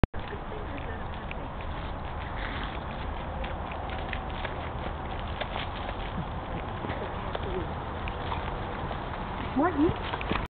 Dalmation eats grass
Morgen has a snack of grass after crossing the bridge